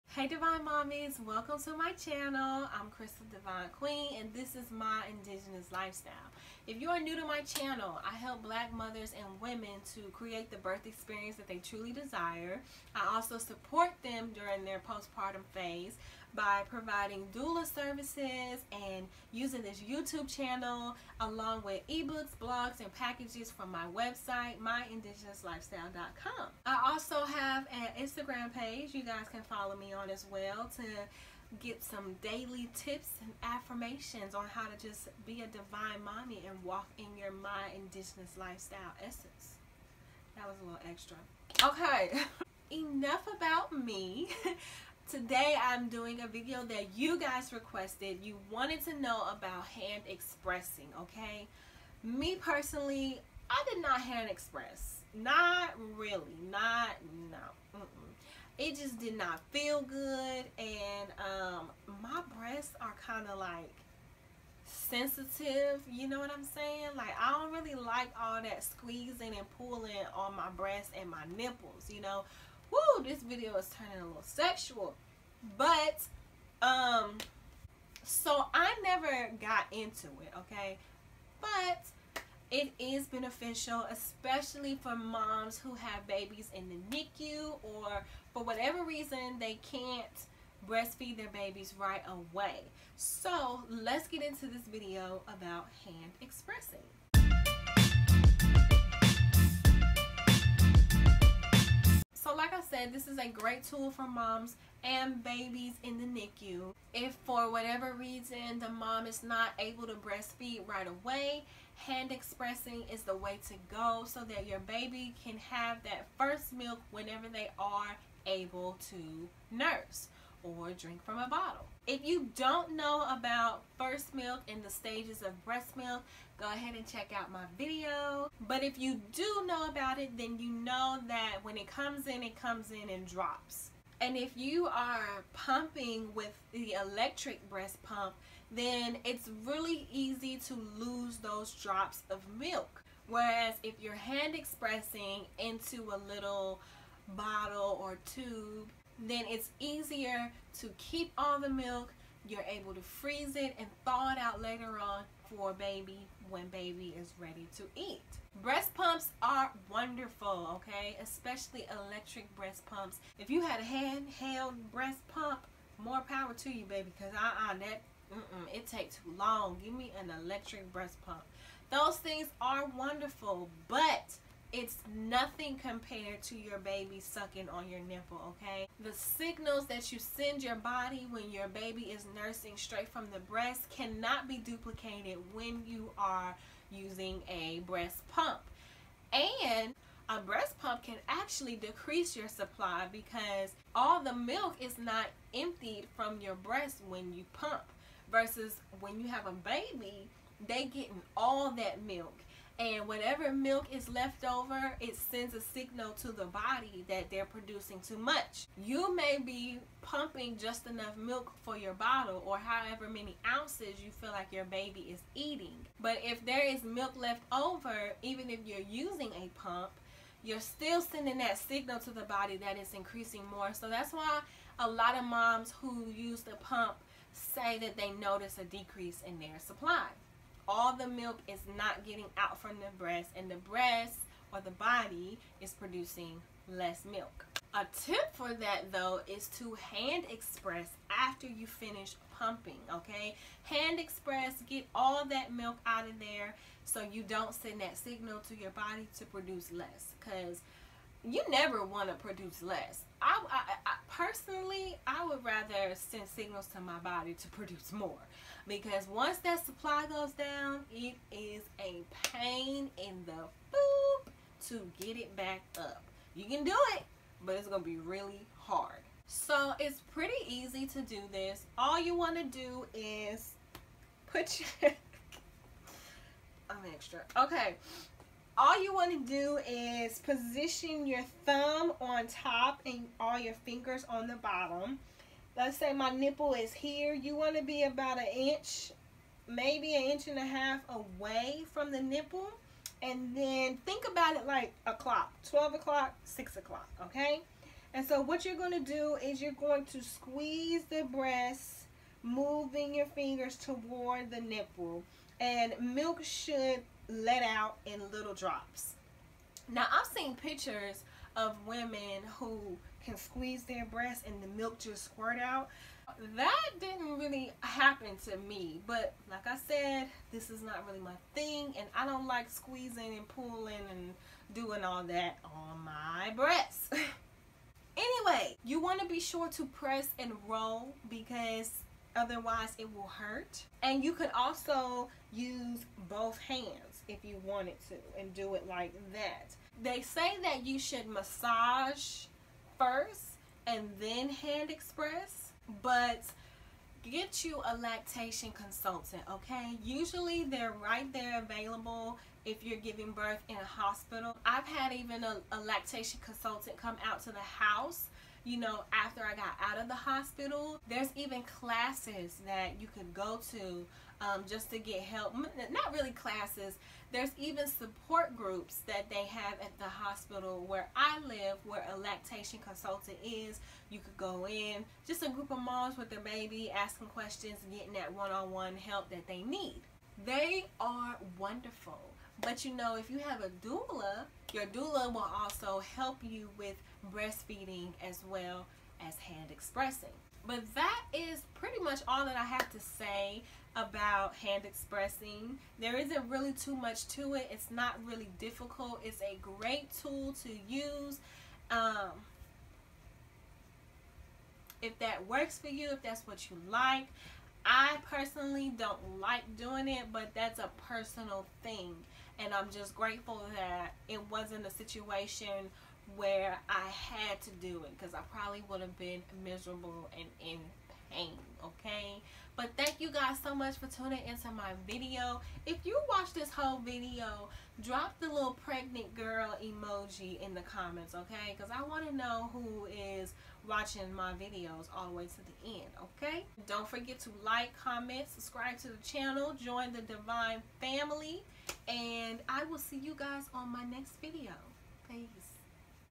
Do You Know How To Hand Express?
Hey Divine Mommies. I put together a video on hand expressing, just like you asked! Enjoy!

AND